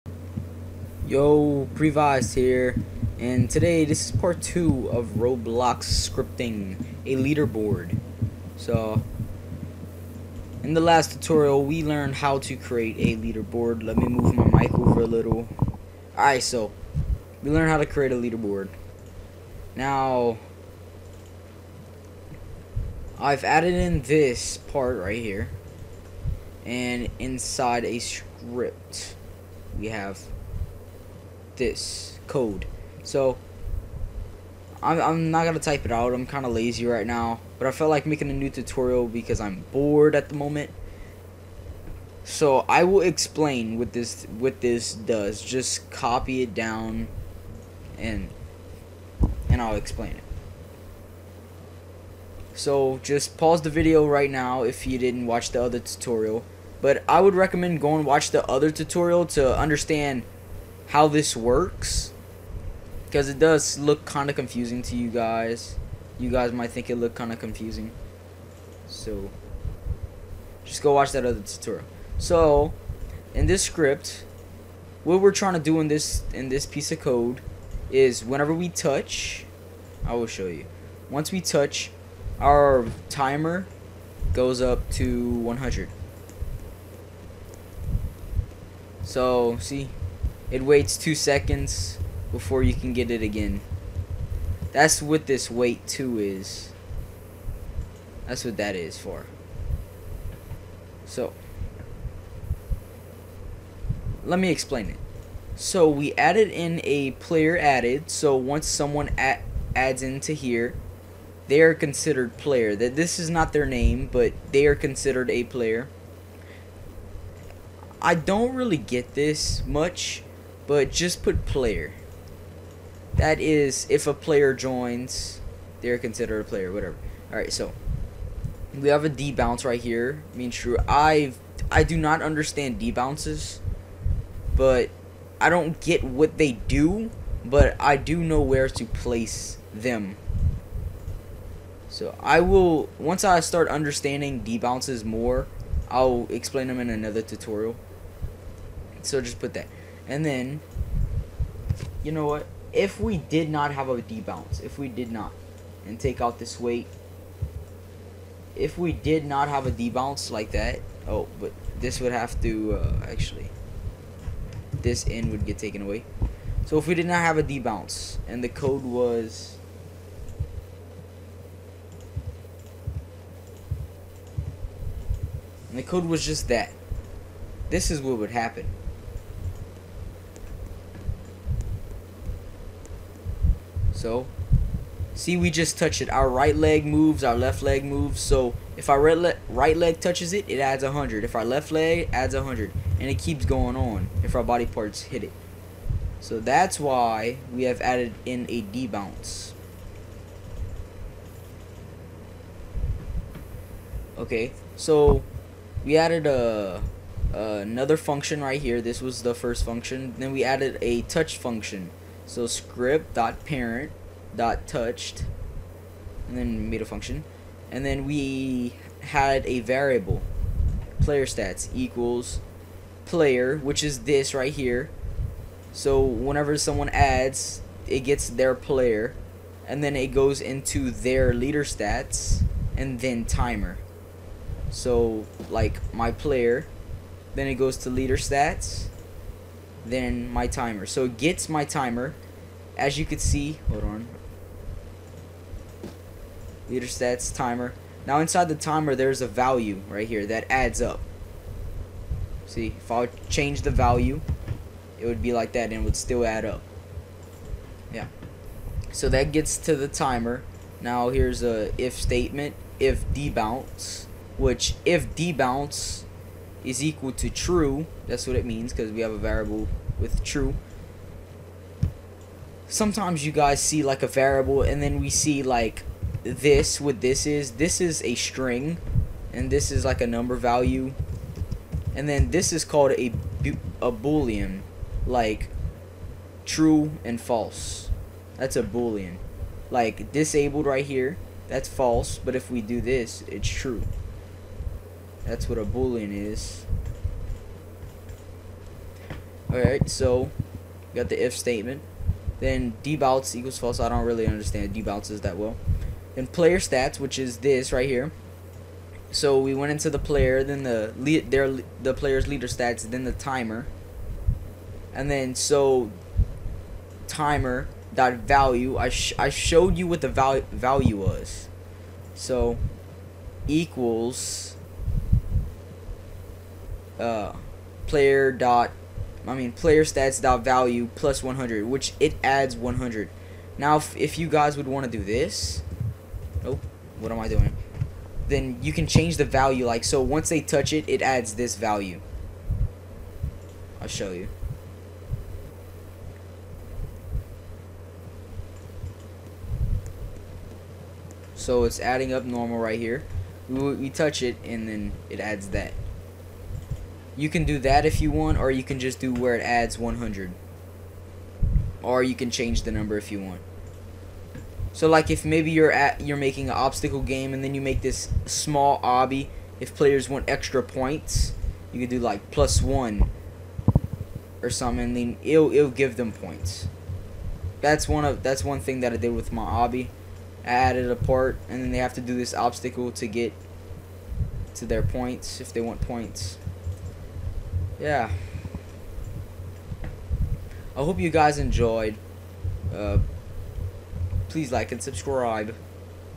ROBLOX Scripting | Leaderboard Pt.2 | Touching a part gives us points
If we touch a part, we get more points.